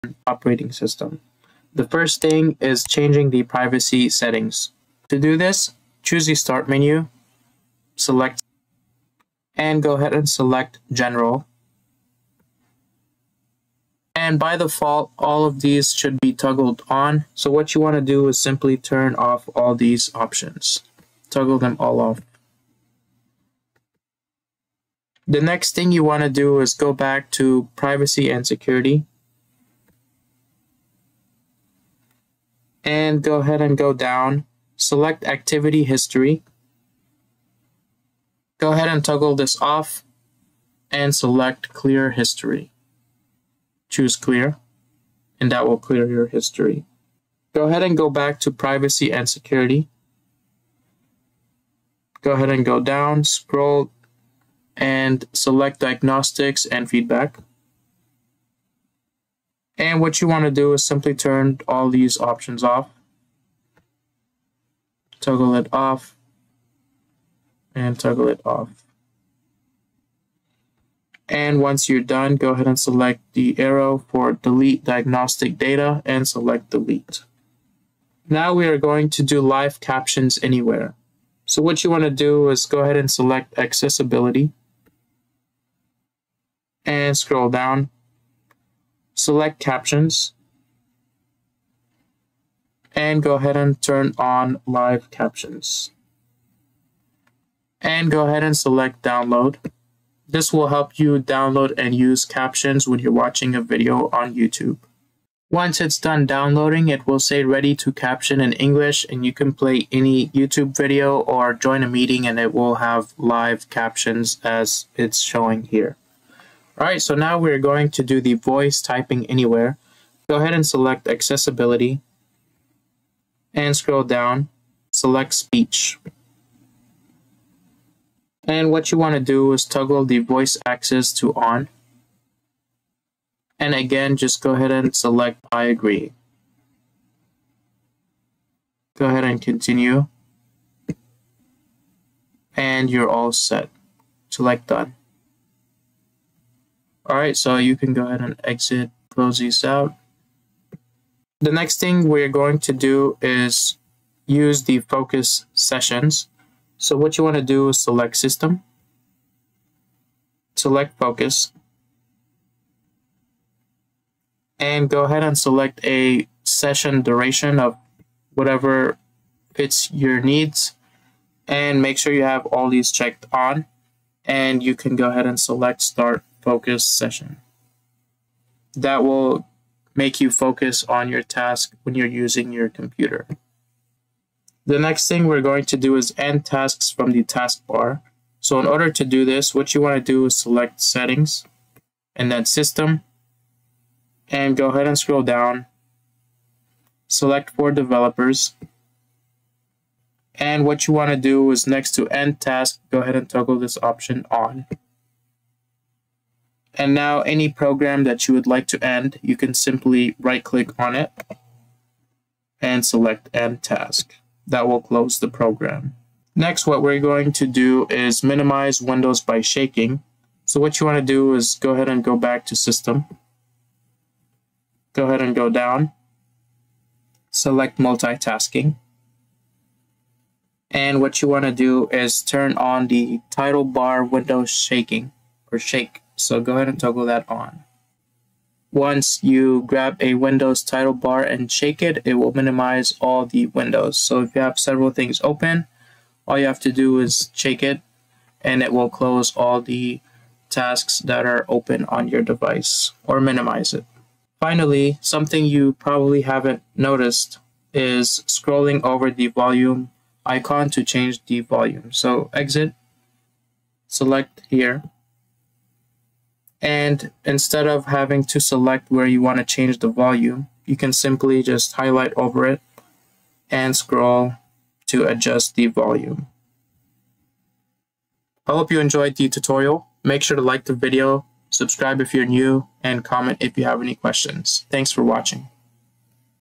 7 Hidden Useful Features YOU DON'T KNOW on Windows 11
7 Hidden Useful Features YOU DON'T KNOW on Windows 11. Watch this 5-minute tutorial to see 7 hidden features you did not know on your Windows 11 Operating System.

**Time Stamps**
0:00 Intro
0:14 Changing Privacy Settings
1:35 Live Captions Anywhere
2:13 Voice Typing Anywhere
2:49 Focus Session
3:22 End Tasks from Taskbar
3:54 Minimize Windows by Shaking
4:35 Change Volume with Scrolling